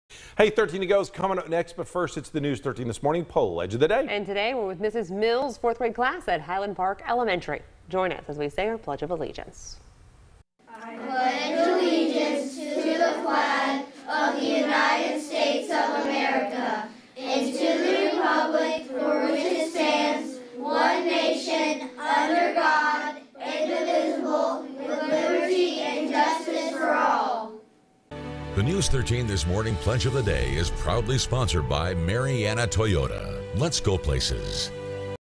The Daily Pledge: Mrs. Mill's 4th Grade Class
The daily pledge was recited by Mrs. Mill's 4th Grade Class at Hiland Park Elementary School!